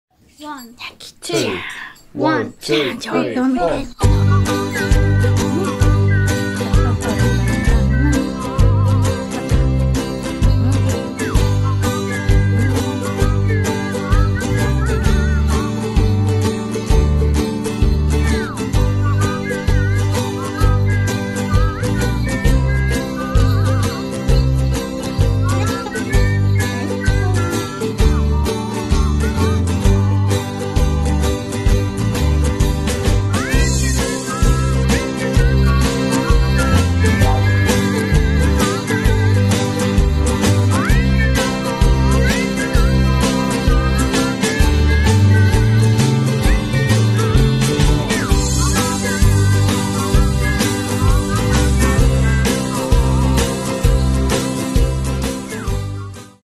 ひなきちゃん今日４歳になりました✨
ひなきちゃんが今日お誕生日で４歳になりました✨